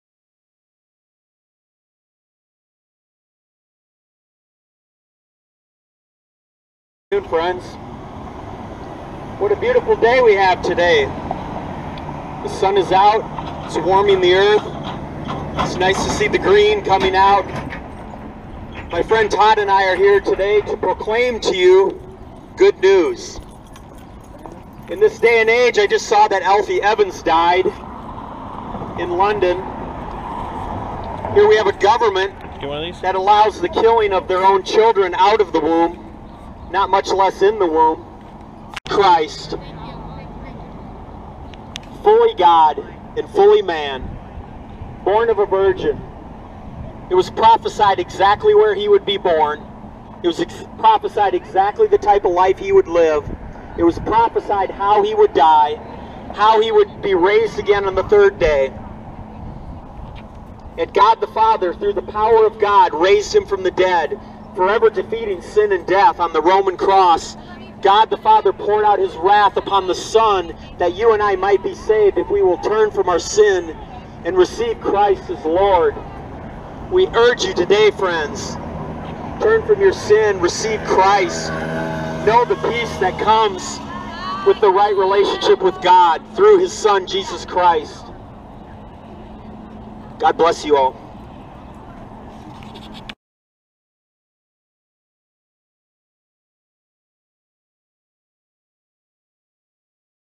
Kevin first Open Air (clips) University of Iowa Saturday April 28th 2018
Please pray for my dear brother Kevin as he is a great blessing to the ministry and to me on a personal level...
I love him immensely...